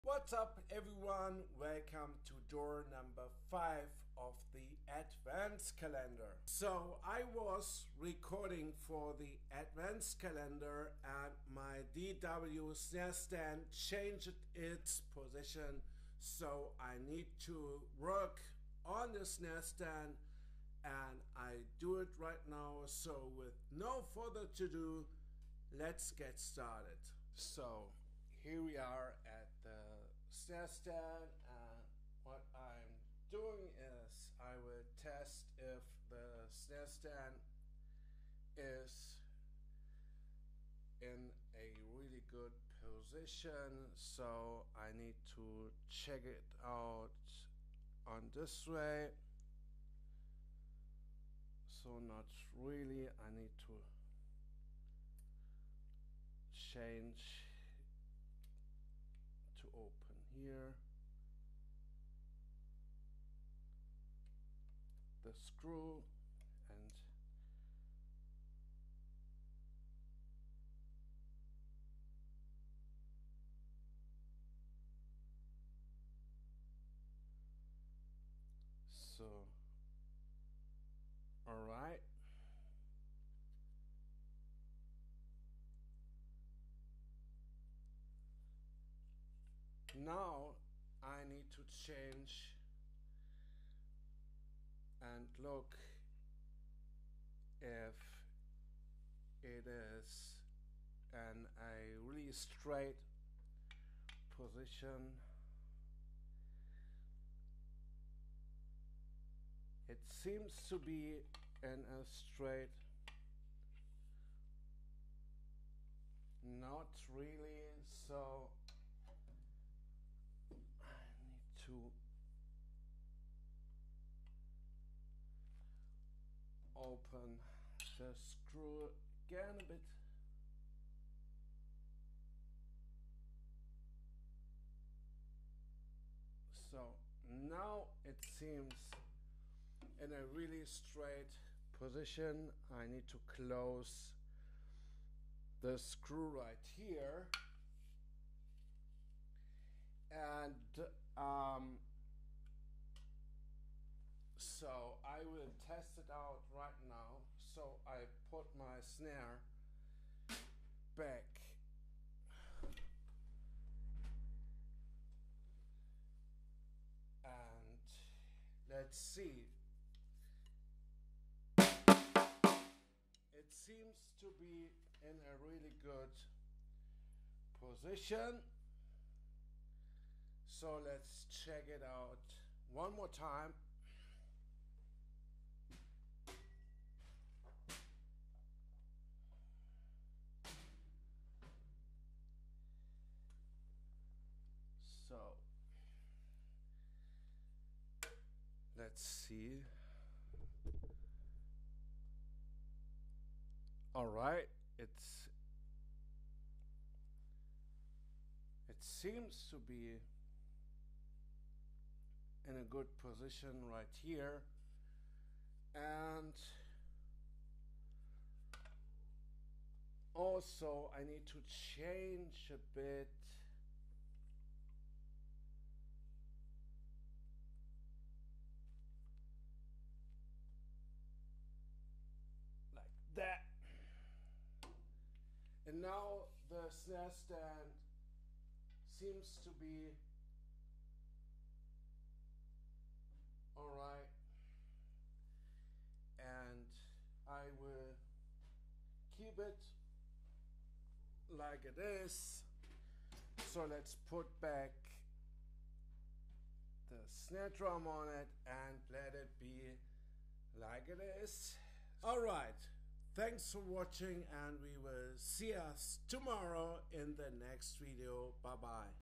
Adventscalendar 2022 Door 5-Working On My DW 9300 Snare Stand
something went wrong with my snarestand while recording for my channel so i worked on it a bit